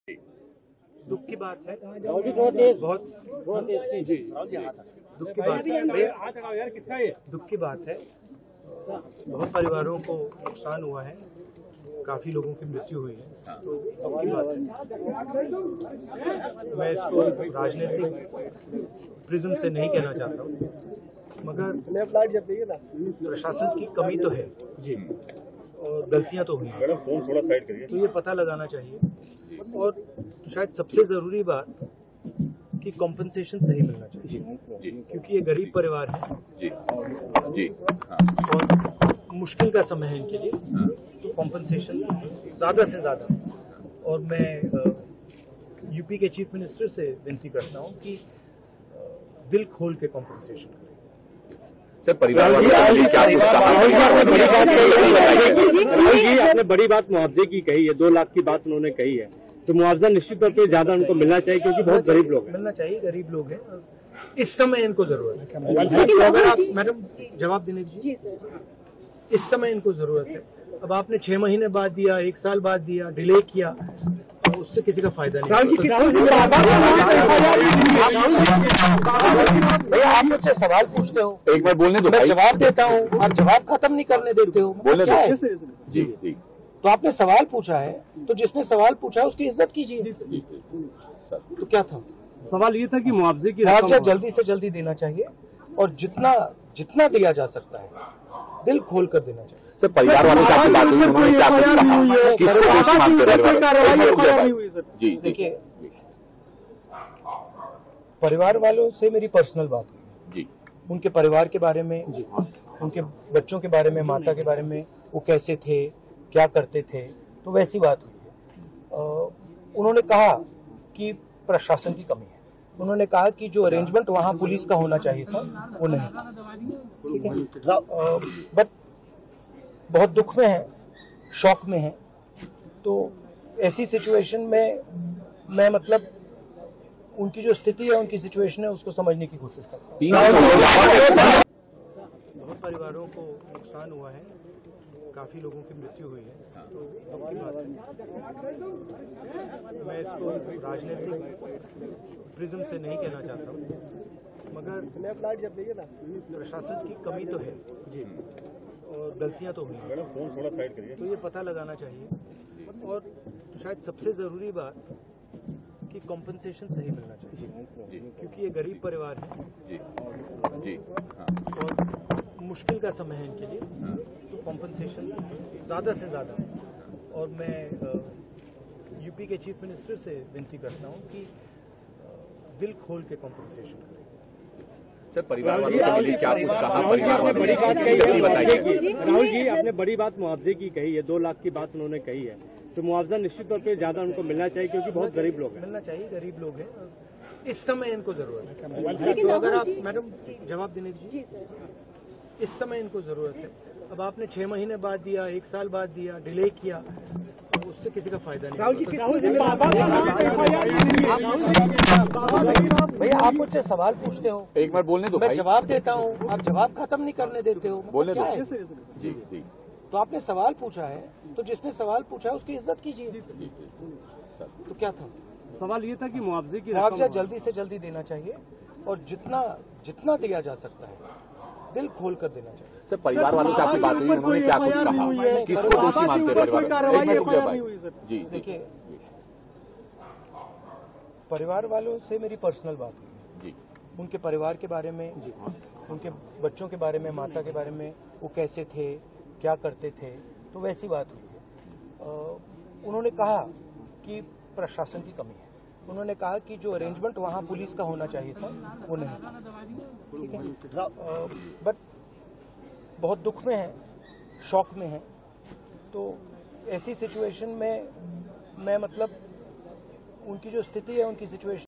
'വളരെ ദരിദ്രരായ കുടുംബങ്ങളാണിവർ, കൂടുതൽ നഷ്ടപരിഹാരം കൊടുക്കണം'; ദുരന്ത ഭൂമിയിൽ രാഹുൽ ഗാന്ധി | Rahul
Rahul Gandhi Speaks to media after visiting Hathras | 'വളരെ ദരിദ്രരായ കുടുംബങ്ങളാണിവർ, കൂടുതൽ നഷ്ടപരിഹാരം കൊടുക്കണം'; ദുരന്ത ഭൂമിയിൽ രാഹുൽ ഗാന്ധി
                ~ED.190~